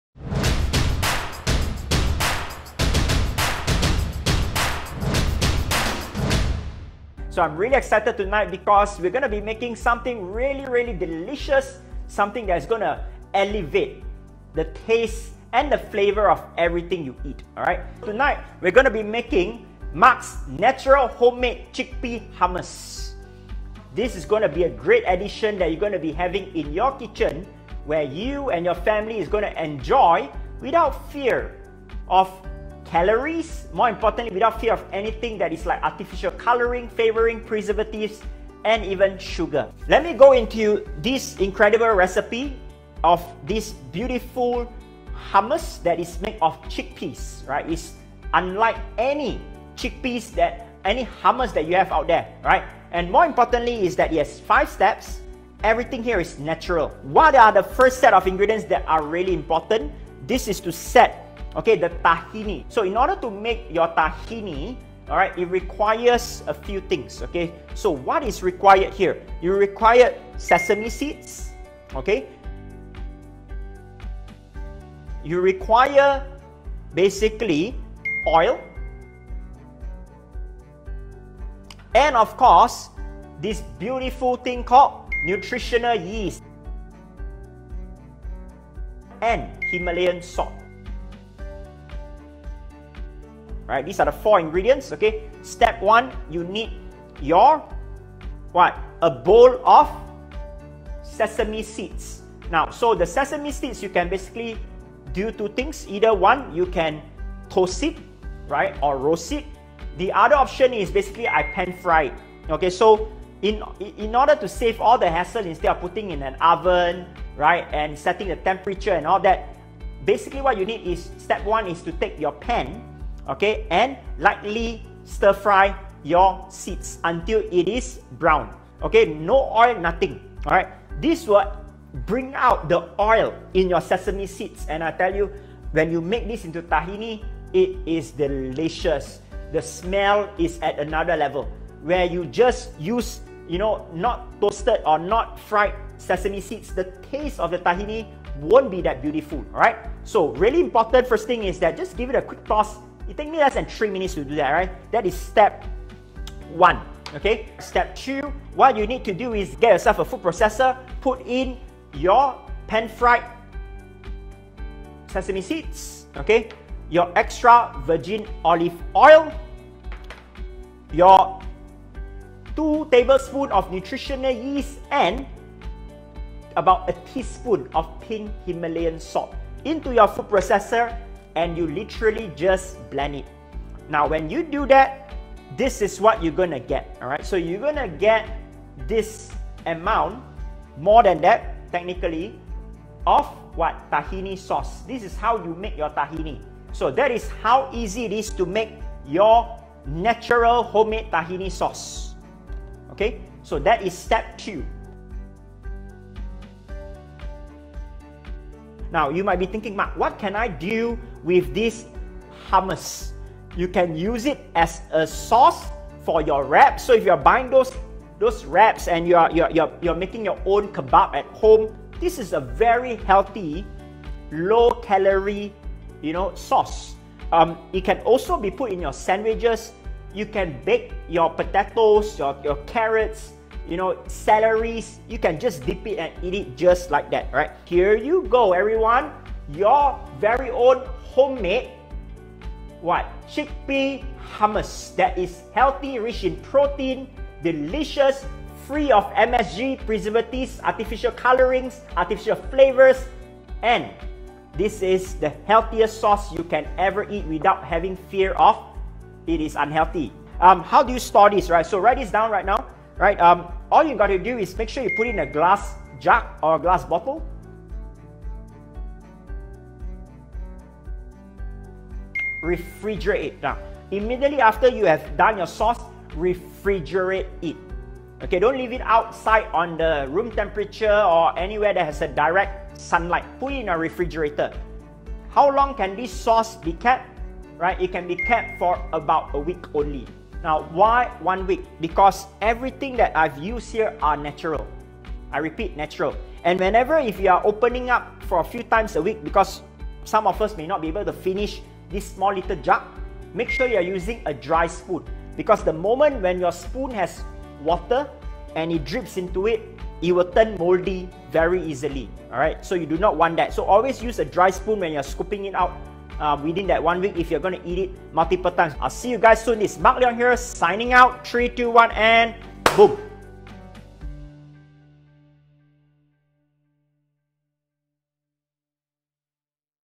How To Make Your Healthy Hummus At Home? | 5 simple steps to make Hummus from real food!
In this video, I share everything you need to know about how you can make your healthy hummus at home using only real food!